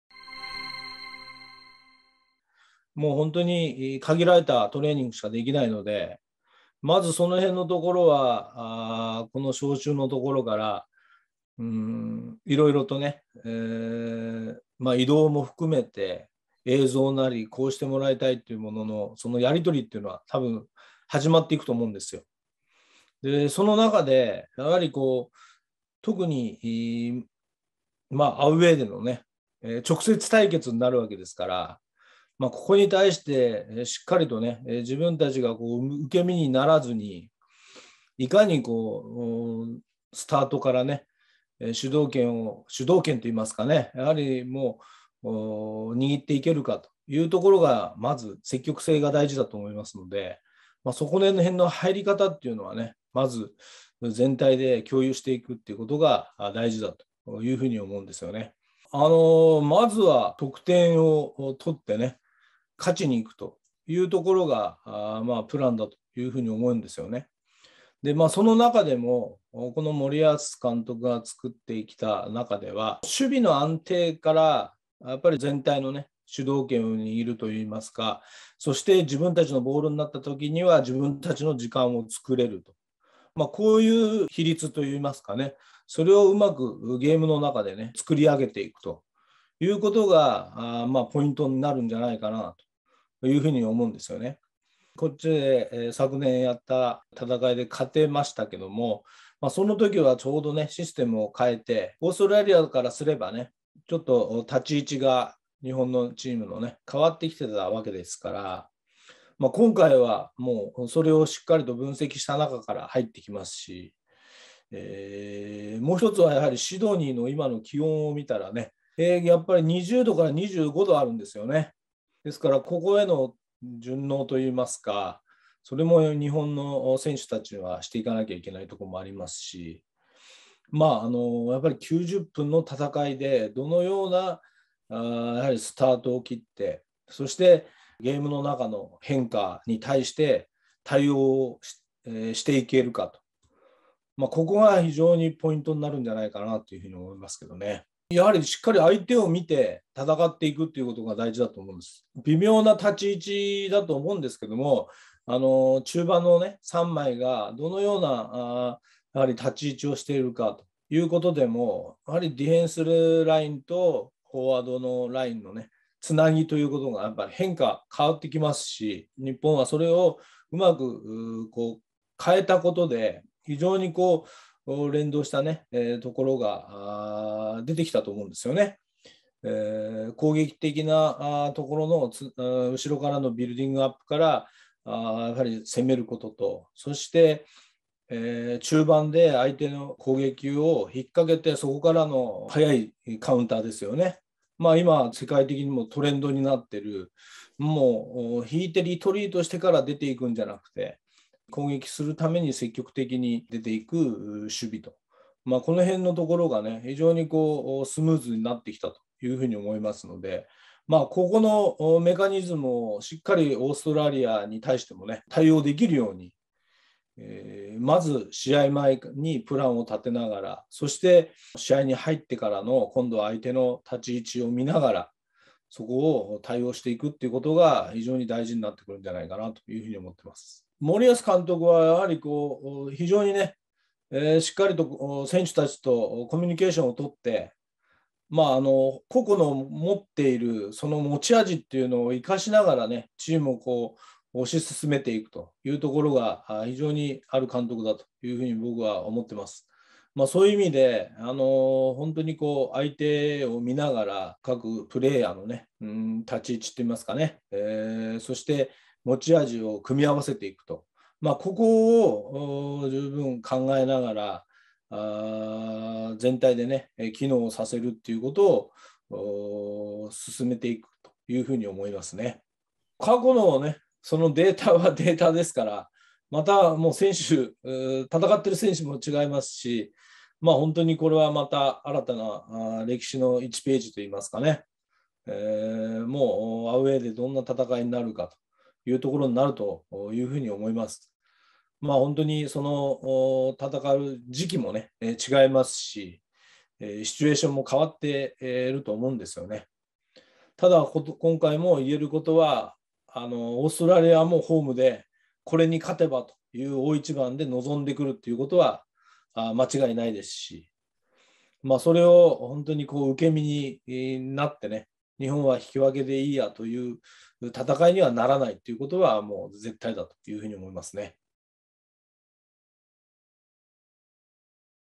【関塚隆が語る】オーストラリアVS日本 運命の豪州戦の結果を大胆予測！【独自見解】
ここでしか聞くことができない元JFA技術委員長の関塚隆が独自見解
あなたはこの予測についてどう思いますか？

3.24オーストラリア戦展望
「引き分け前提の戦いにはならない」
元JFA技術委員長の関塚隆が独自見解。

00:02 どんなアプローチでオーストラリア戦に臨むべきか？

00:58 ベストな試合プランは？

02:28 日本を相当研究してくるオーストラリアにどう対応すべきか？

03:50 森保監督の采配で期待したいところは？

04:33 過去のＷ杯予選ではアウェーで オーストラリアに勝てていないが…

第一弾はこちら
【運命の豪州戦】三笘、旗手、久保、上田…。関塚隆が語る「招集メンバー27名の印象」

●サッカーダイジェスト関連本の販売サイト